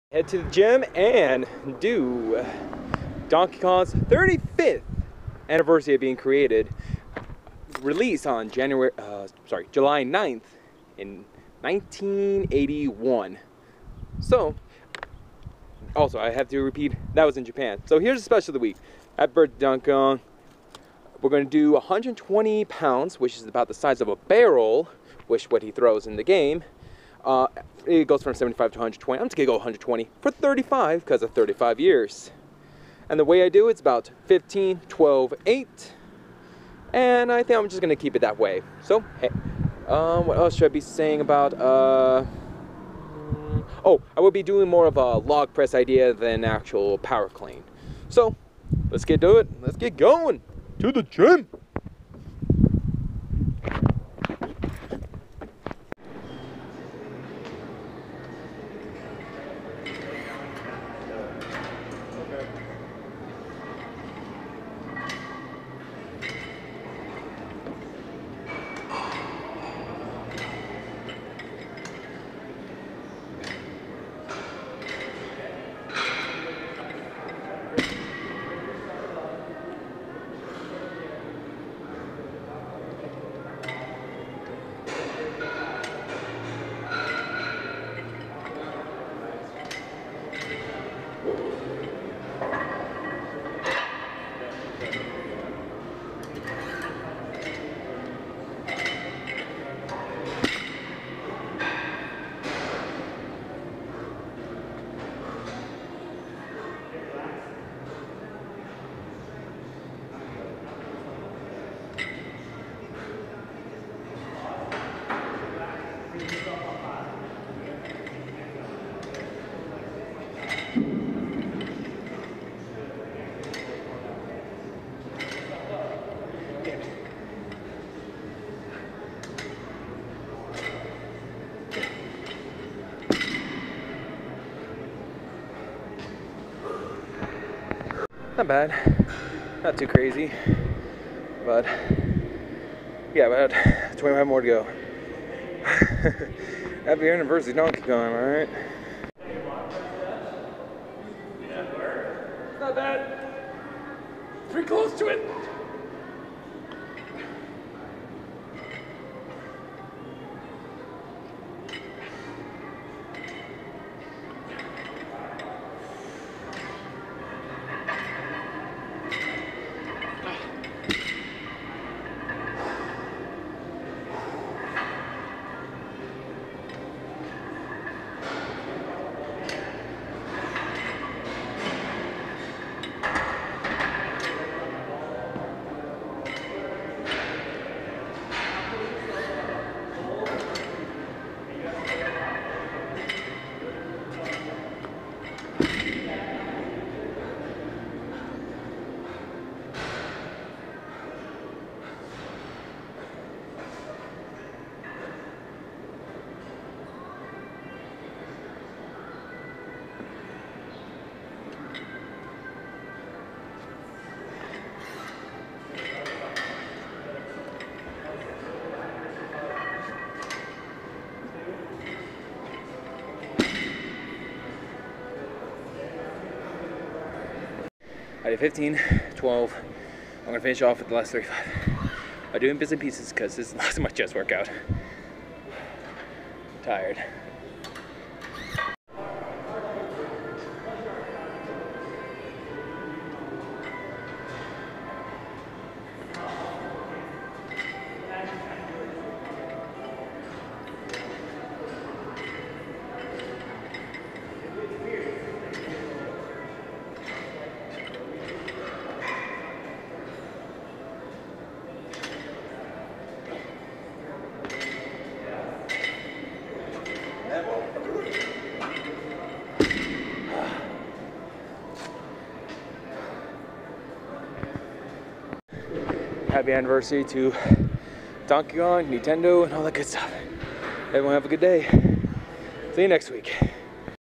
Donkey Kong Lifting Challenge! Happy Birthday DK
It is the Japanese release of Donkey Kong original Arcade game. Lets Go Bananas in this Challenge.
Since a average empty barrel is 75 to 120 pounds, I chose to go heavy!.

120 Ibs X 35 years (repes)